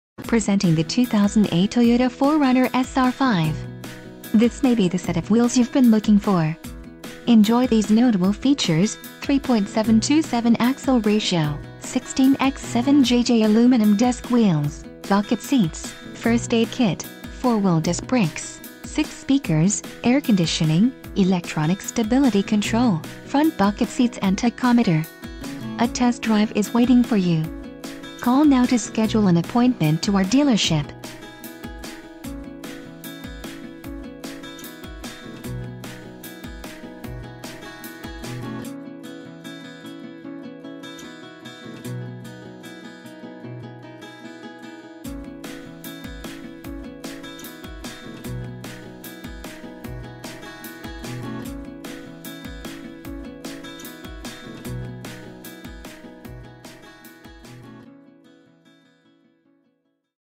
2008 Toyota 4Runner SR5 - Beaver Toyota - Call or Text No...
2008 Toyota 4Runner SR5 Condition: Used Mileage: 113311 Transmission: 5-Speed Automatic with Overdrive Doors: 4 Stock: 109954AA Cylinders: 6